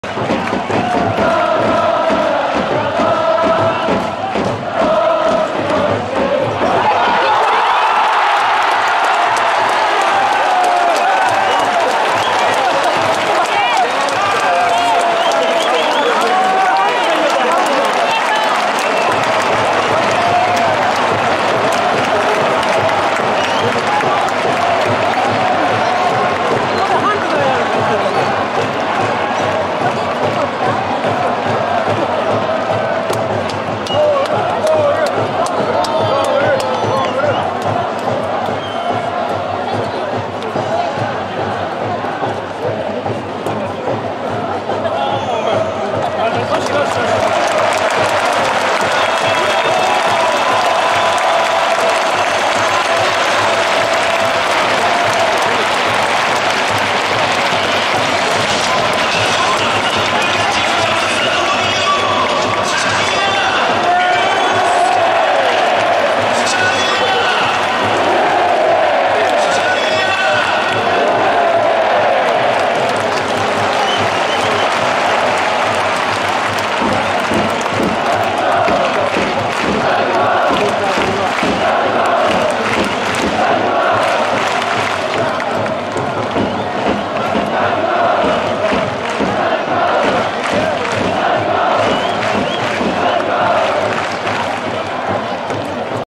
【後半26分】チャジの勝ち越しゴール時の、Ｂ６サポの歓喜⚽⚽ 2023.06.04 J1 第16節 京都サンガfc 戦
明治安田生命 DAY